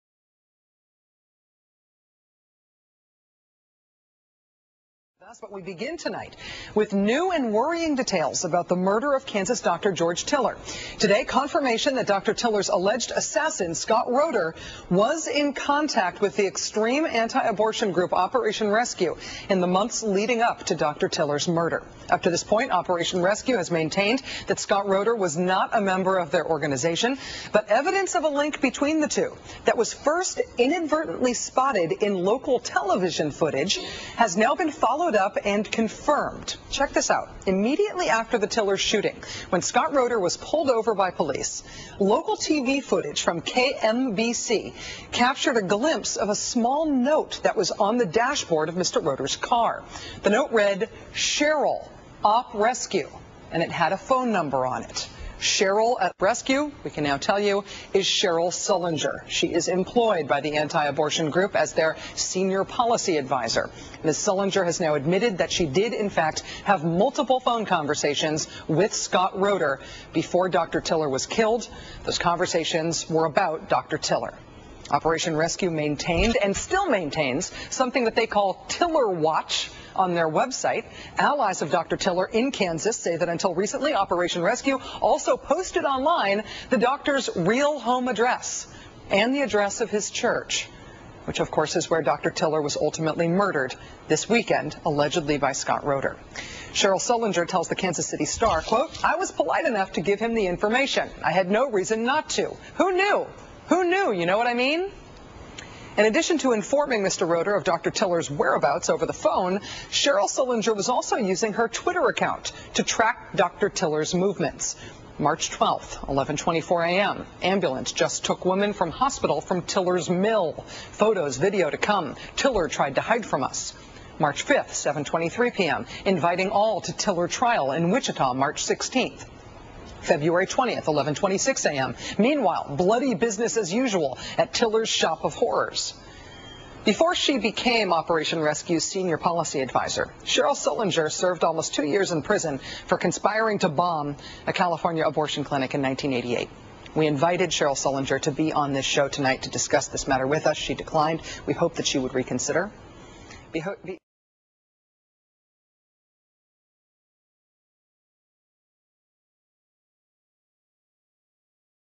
Evidence Connecting Scott Roeder to Operation Rescue Found in Roder's Car
Rachel Maddow unveils video evidence from KMBC TV in Kansas City of a note found in Scott Roeder's car at the time of his arrest for the murder of Dr. Geroge Tiller that read "Cheryl Op Rescue" and then a phone number.  Cheryl Sullenger, Senior Policy Advisor for Operation Rescue, has admitted to having several conversations with Roeder about Dr. Tiller.